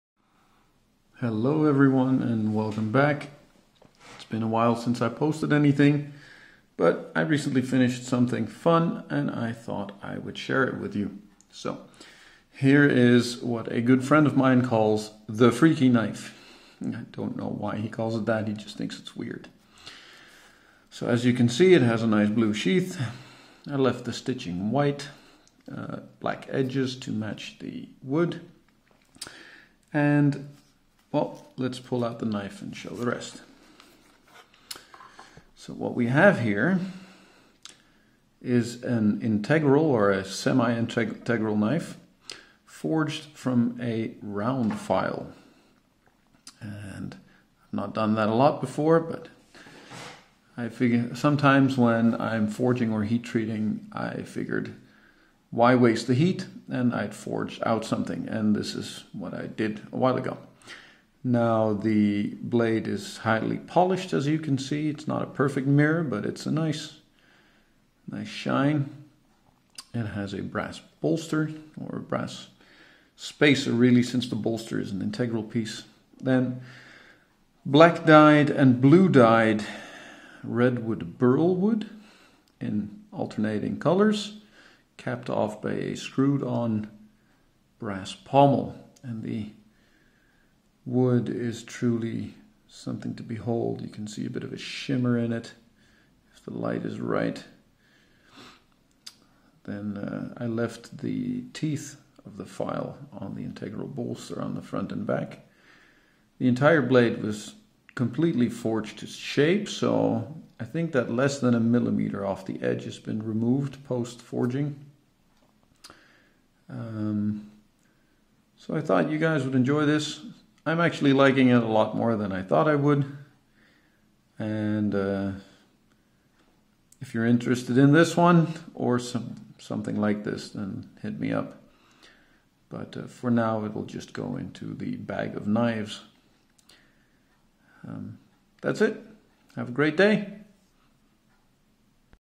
Semi-integral file knife.
Semi integral knife forged from an old round file.

blade polished and buffed.

handle from redwood burl, stabilised and dyed blue and black.

simple right handed blue sheath.